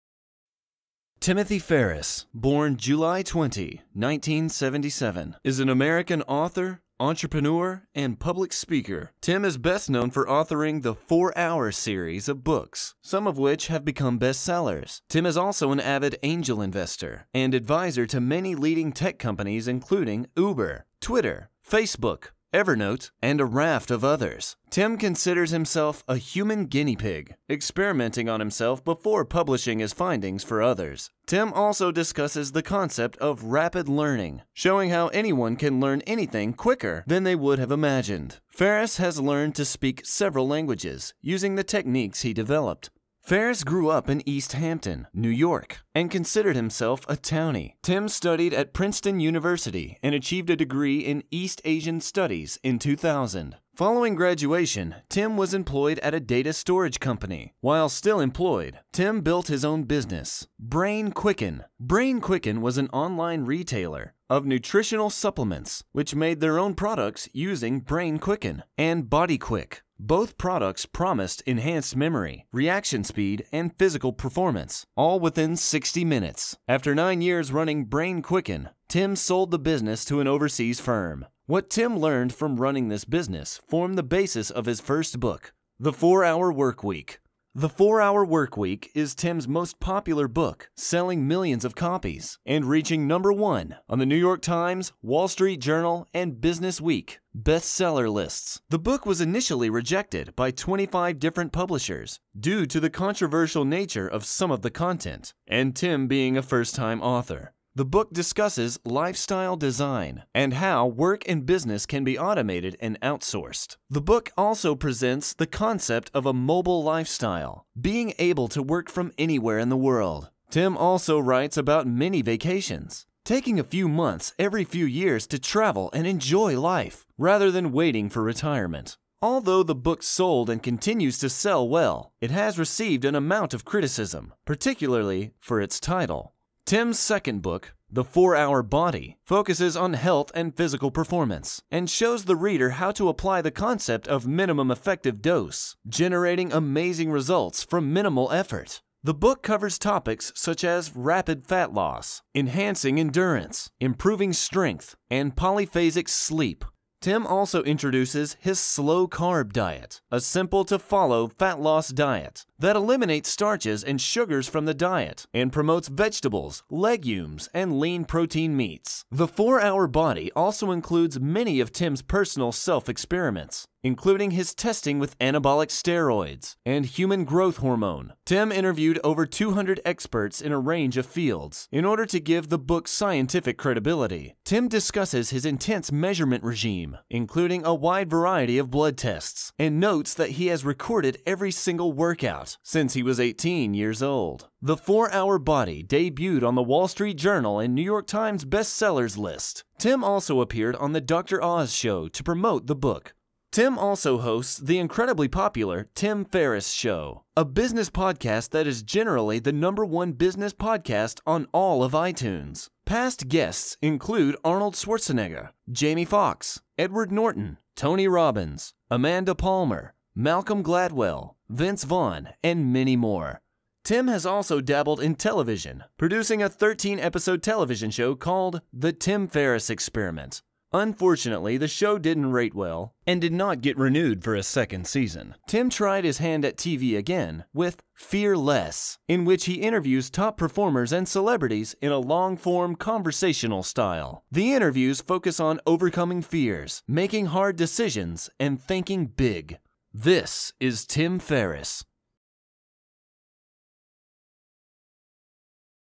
Who Is Tim Ferriss?
Timothy Ferriss, born July 20, 1977, is an American author, entrepreneur, and public speaker. Tim is best known for authoring the "4 Hour" series of books, some of which have become best sellers. Tim is also an avid angel investor and adviser to many leading tech companies including Uber, Twitter, Facebook, Evernote and a raft of others. Tim considers himself a "human guinea pig", experimenting on himself before publishing his findings for others. Tim also discusses the concept of 'rapid-learning', showing how anyone can learn anything quicker than they would have imagined. Ferriss has learned to speak several languages using the techniques he developed.

Ferriss grew up in East Hampton, New York and considered himself a 'townie'. Tim studied at Princeton University and achieved a degree in East Asian Studies in 2000. Following graduation Tim was employed at a data storage company. While still employed, Tim build his own business, BrainQUICKEN. BrainQUICKEN was an online retailer of nutritional supplements, which made their own products including BrainQuicken and BodyQuick. Both products promised enhanced memory, reaction speed and physical performance, all within sixty minutes. After nine years running BrainQUICKEN, Tim sold the business to an overseas firm. What Tim learned from running this business formed the basis for his first book, 'The 4-hour work week'.

'The 4 hour work week', is Tim's most popular book selling millions of copies and reaching number one on the New York Times, Wall Street Journal and BusinessWeek bestseller lists. The book was initially rejected by 25 different publishers, due to the controversial nature of some of the content and Tim being a first time author. The book discusses 'lifestyle design' and how work and business can be automated and outsourced. The book also presents the concept of a 'mobile lifestyle', being able to work from anywhere in the world. Tim also writes about 'mini-vacations', taking a few months every few years to travel and enjoy life, rather than waiting for retirement.
Although the book sold and continues to sell well, it has received an amount of criticism, particularly for its title.

Tim's second book 'The 4 hour body' focuses on health and physical performance, and shows the reader how to apply the concept of 'minimum effective dose', generating amazing results from minimal effort. The book covers topics such as rapid fat loss, enhancing endurance, improving strength and polyphasic sleep. Tim also introduces his 'slow carb diet', a simple to follow fat loss diet that eliminates starches and sugars from the diet and promotes vegetables, legumes and lean protein meats. 'The 4 hour body' also includes many of Tim's personal self-experiments, including his testing with anabolic steroids and human growth hormone. Tim interviewed over 200 experts in a range of fields in order to give the book scientific credibility. Tim discusses his intense measurement regime including a wide variety of blood tests, and notes that he has recorded every single workout since he was 18 years old. 'The 4 hour body' debuted on the Wall Street Journal and The New York Times best sellers list. Tim also appeared on The Dr. Oz Show to promote the book.

Tim also hosts the incredibly popular 'Tim Ferriss Show', a business podcast that is generally the number one business podcast on all of iTunes. Past guests include Arnold Schwarzenegger, Jamie Foxx, Edward Norton, Tony Robbins, Amanda Palmer, Malcolm Gladwell, Vince Vaughn and many more.

Tim has also dabbled in television producing a 13 episode television show called 'The Tim Ferriss Experiment'. Unfortunately the show didn't rate well and did not get renewed for a second season. Tim tried his hand at TV again with 'Fear-less' in which he interviews top performers and celebrities in a long-form conversational style. The interviews focus on overcoming fears, making hard decisions and thinking big.

This is Tim Ferriss.